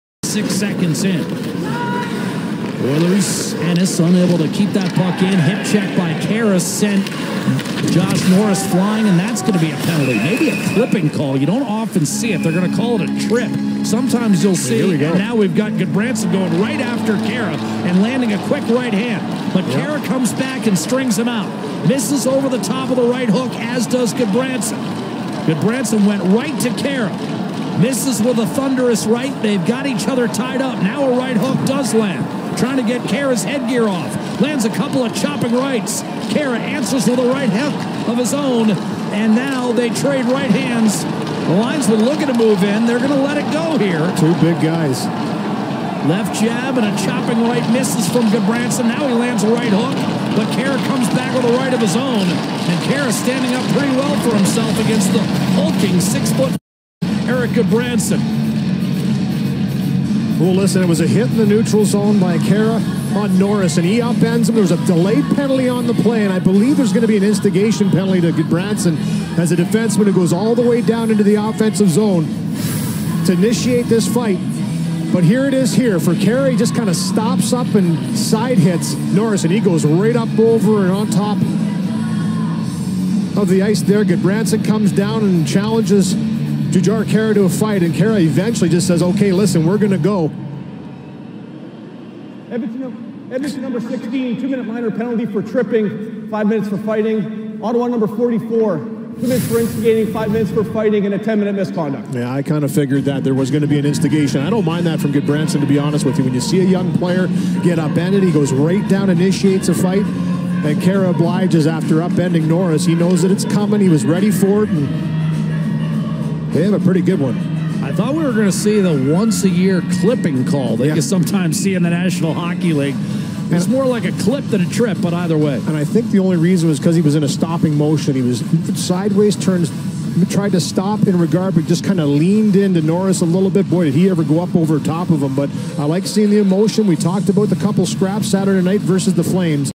Jujhar Khaira Left Bloodied After Tilt With Erik Gudbranson
Khaira thows a HUGE hip check at center ice and clearly Gudbranson didn't approve of it as he skates directly into the Oilers zone after Khaira to have a word.